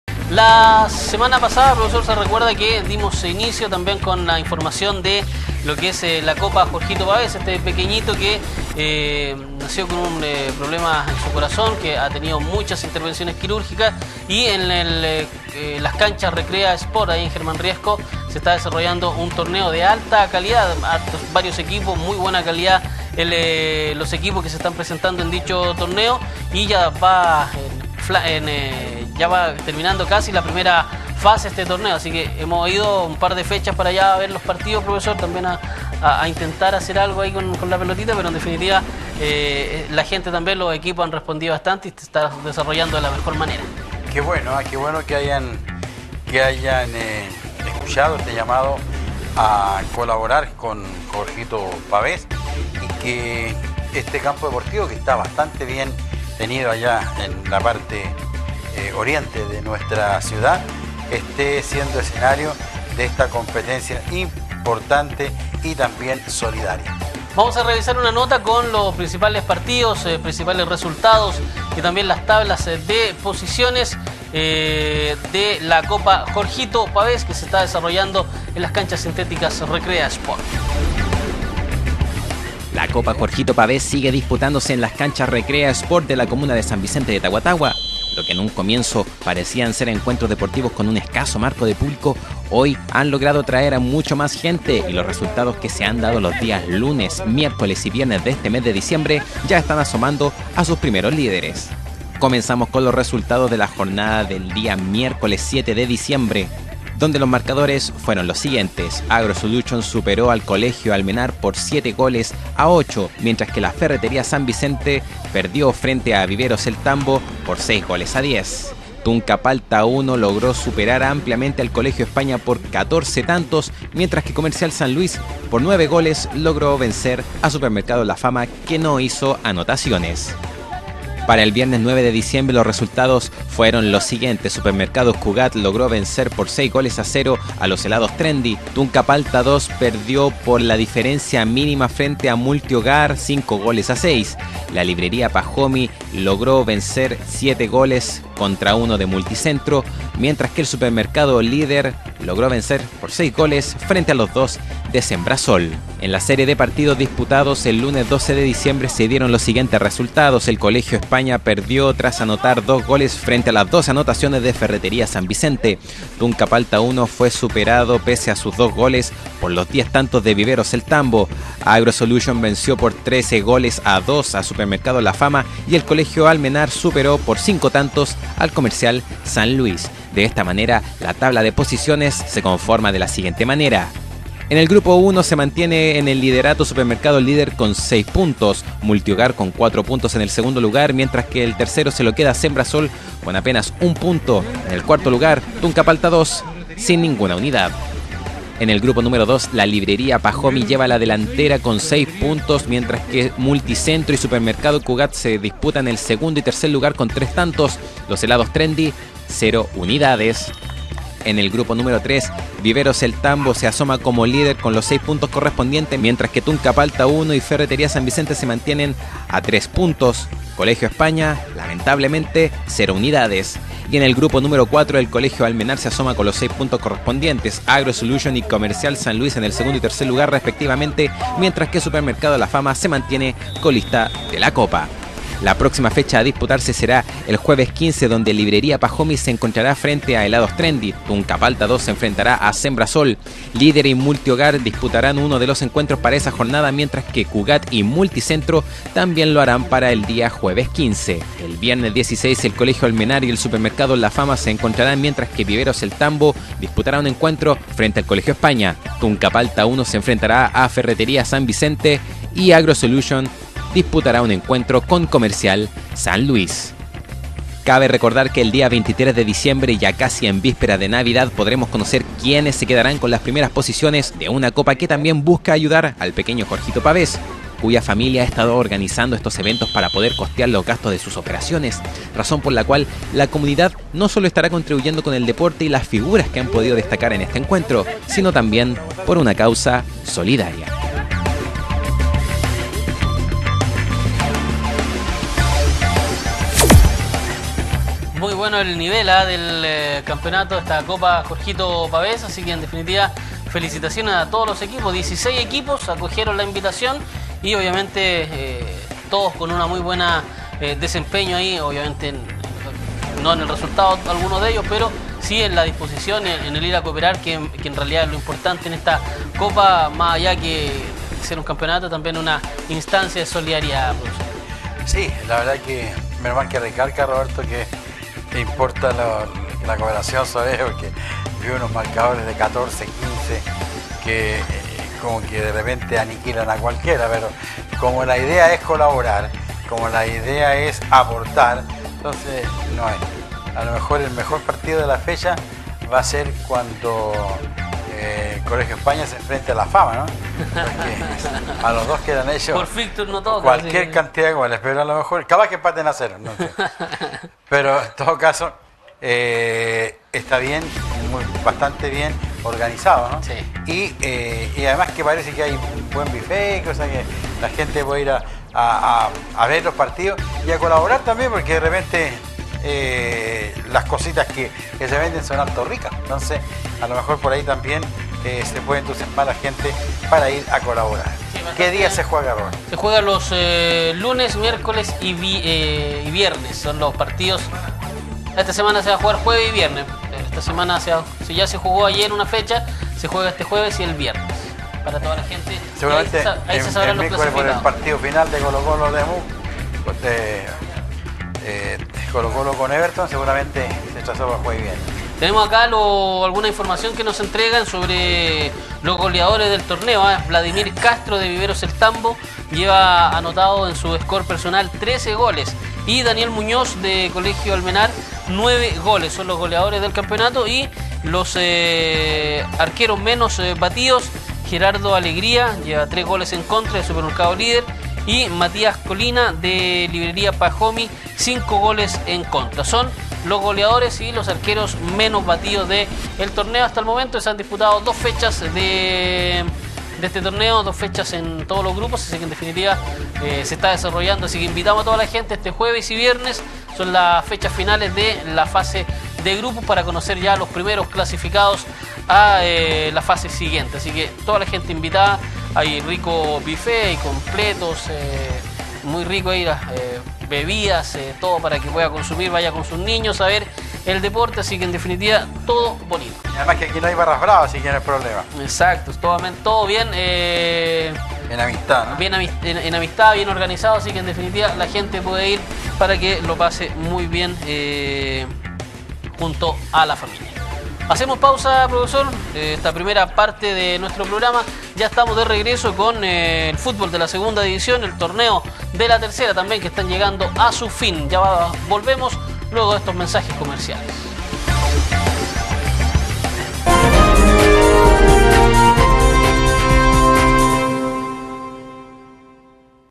Ya asoman los primeros lugares de la Copa Jorgito Pavez
La Copa Jorgito Pavez sigue disputándose en las canchas REKREASPORT de la comuna de San Vicente de Tagua Tagua, lo que en un comienzo parecían ser encuentros deportivos con un escaso marco de público hoy han logrado atraer a mucho más gente y los resultados que se han dado los días lunes, miércoles y viernes de este mes de diciembre ya están asomando a sus primeros líderes.